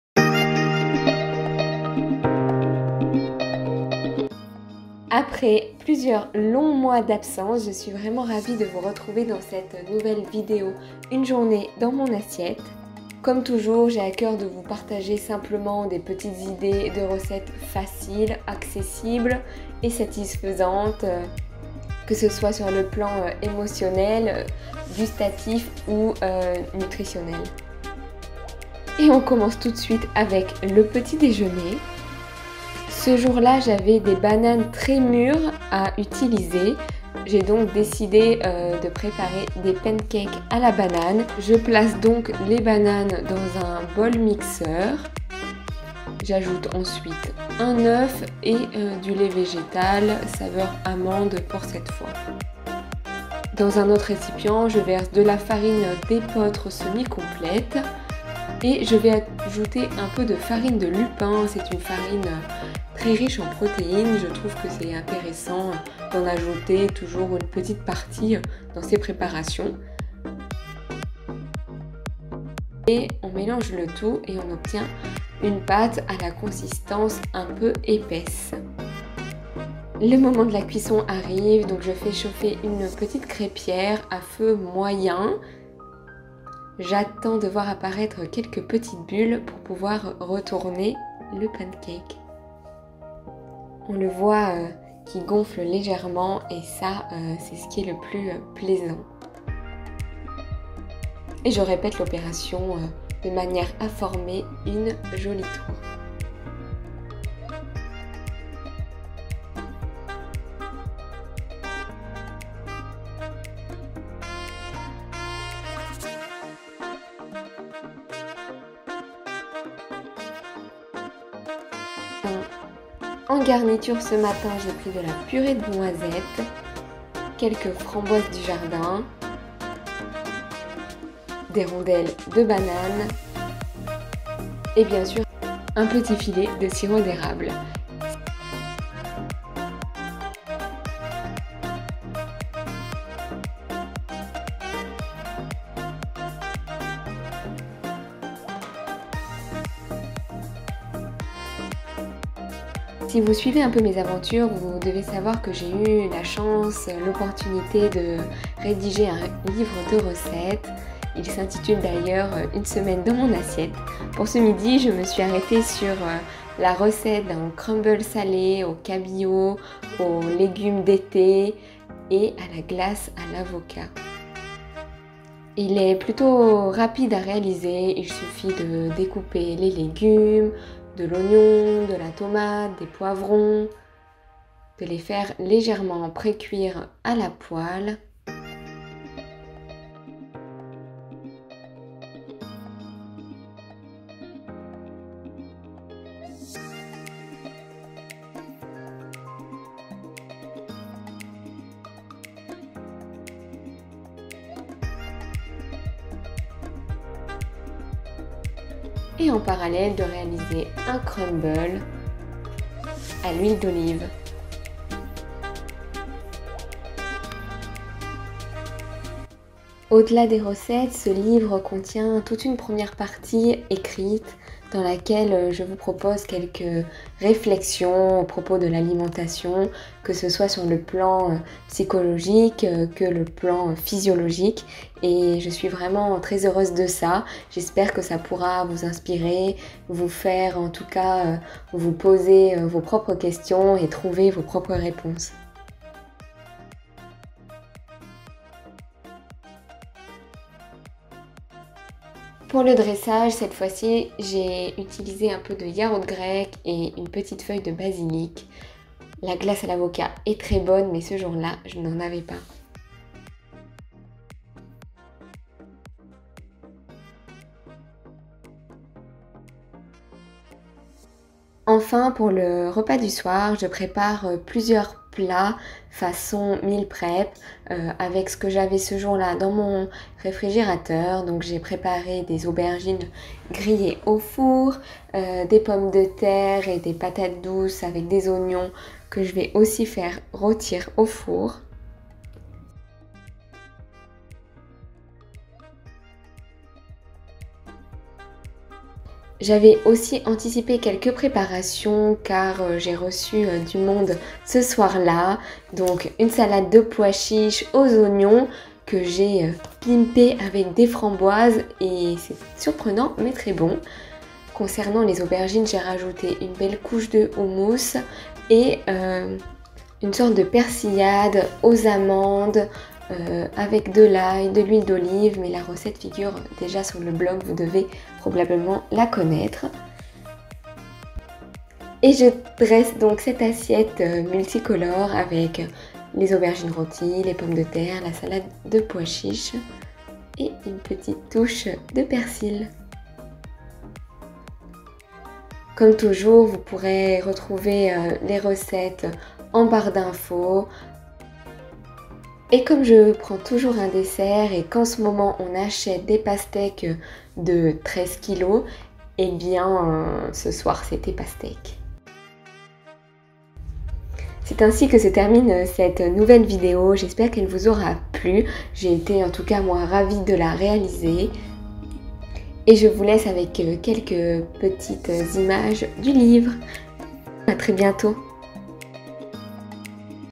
Une journée dans mon assiette / Episode 12.
Très heureuse de vous retrouver dans une nouvelle vidéo une journée dans mon assiette après plusieurs mois d’absence !

Comme toujours l'idée est de partager des recettes de tous les jours simples & satisfaisantes. Prendre du plaisir à créer et déguster.

Les recettes :

1 / PANCAKES BANANE / 10 larges pancakes
- 2 bananes bien mûres (220 g)
- 1 œuf (large)
- 150 ml de lait végétal
- 150 g de farine de blé semi-complète
- 40 g de farine de lupin
- 4 g de poudre à lever
- 1 pincée de sel
- 1 cc d'arôme vanille
= cuisson sur feu doux afin d'avoir une cuisson uniforme à l’intérieur.

2 / CRUMBLE DU SOLEIL  ( Recette issue du livre)

3 / ASSIETTE COMPOSÉE

Salade de pois chiches aux framboises
1 boite de pois chiche
1 petit avocat
1 tasse de framboises
1 tige d'oignon nouveau
1cc de moutarde
2cs d'huile de sésame
1cs de vinaigre de cidre
Une touche de persil frais

Patate douce ; pomme de terre et oignons rôtis
1 patate douce
2 pommes de terre
1 oignon
2 cs d'huile d'olive
sel poivre et cumin.
Cuisson : 40 minutes 180°

Aubergines aux houmous et persillade aux amandes
2 aubergines (précuites : cuisson 40 minutes 170°)
4-6 cs de houmous
1 tasse d'amande
2 poignées de persil frais
1 gousse d'ail
2 cs d'huile d'olive
sel & poivre

Au plaisir de vous retrouver sur mon compte instagram @HEALTHYCLEMSY.

 Musique proposée par La Musique Libre